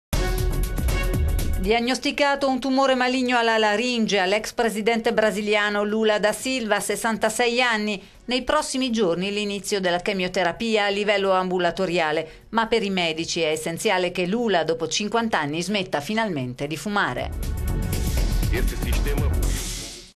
Brasile: Lula da Silva ha un tumore alla laringe
Diagnosticato un tumore maligno alla laringe all'ex presidente brasiliano Luiz Inacio Lula da Silva, 66 anni. Nei prossimi giorni l'inizio della chemioterapia a livello ambulatoriale. Ma per i medici è essenziale che Lula, dopo cinquant'anni, smetta finalmente di fumare.